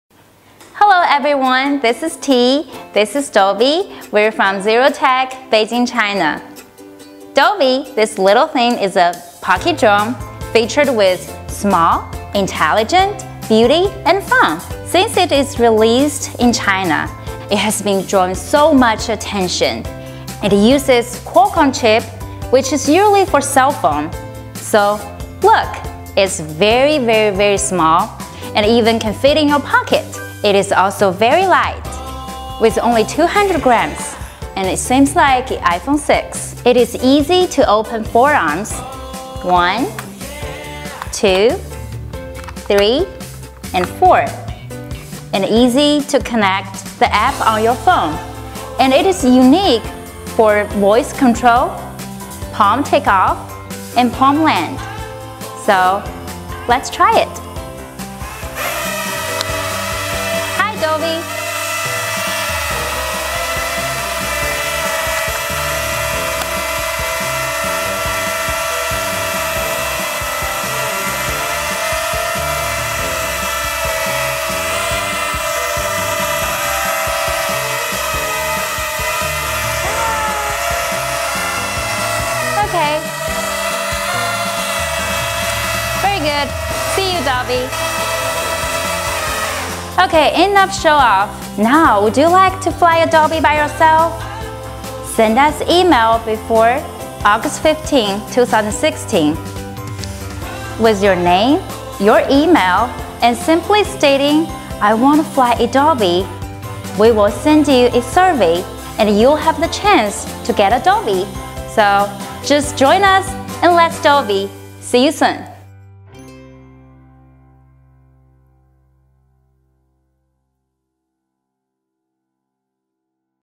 Take the advantage!!! Are you the next one to fly a ZEROTECH DOBBY?
DOBBY is the first pocket sized mini selfie drone that weighs only 199g, it can capture 4K images and 1080p HD video.